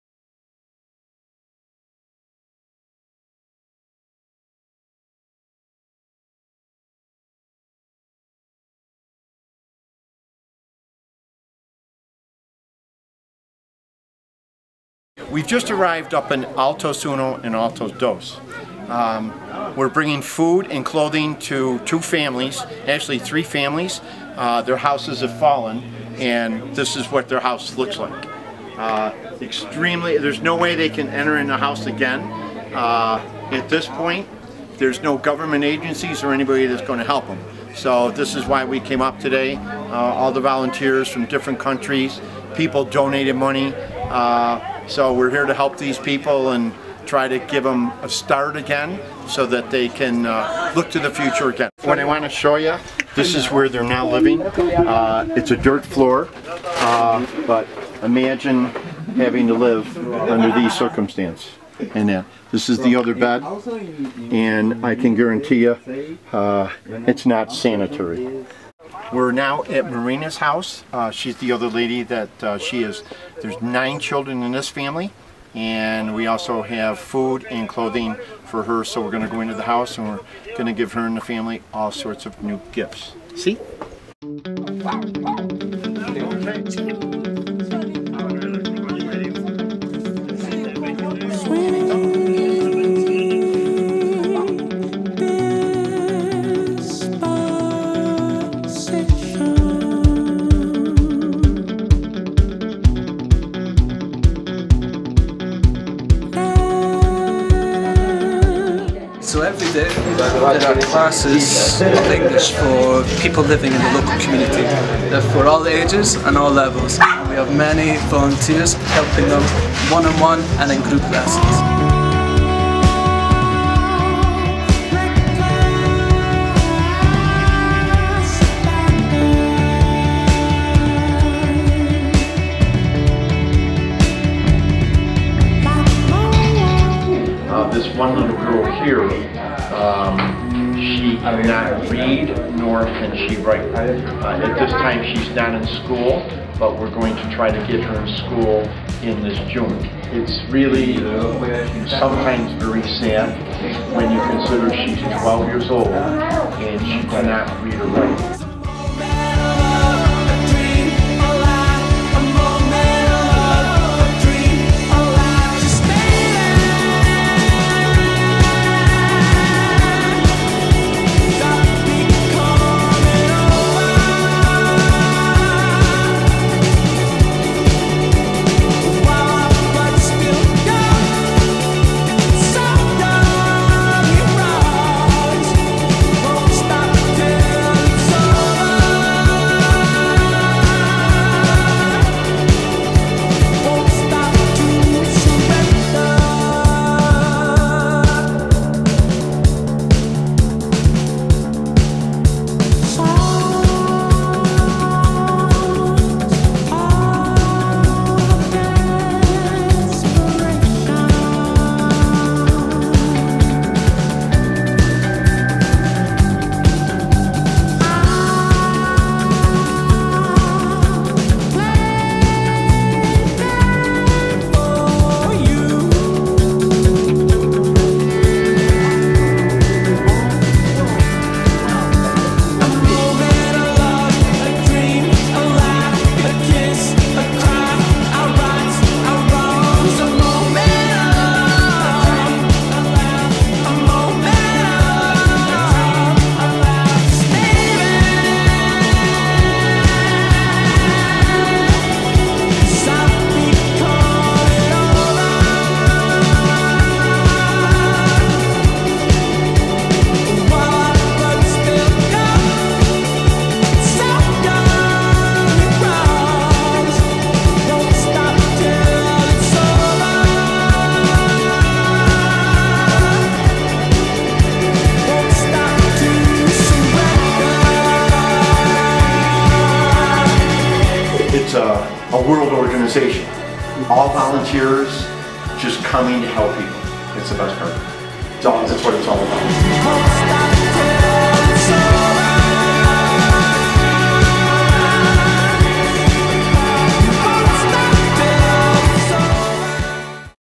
Angeles De Medellin Promotional Video
A Short Promotional film by Felix Nelson & Leva Kwestani following the wonderful work of Marcos Kaseman and his Angeles De Medellin Foundation. Angeles de Medellin is a program helping poor and displaced children and families in Medellin, Colombia. For more information on the fantastic work they do, please

Credits:
Film by Felix Nelson
Editing by Leva Kwestany
Filming by Felix Nelson & Oliver Davis

Special Thanks to Marcos and all the Volunteers for all the assistance in making this film possible. Keep up the great work also!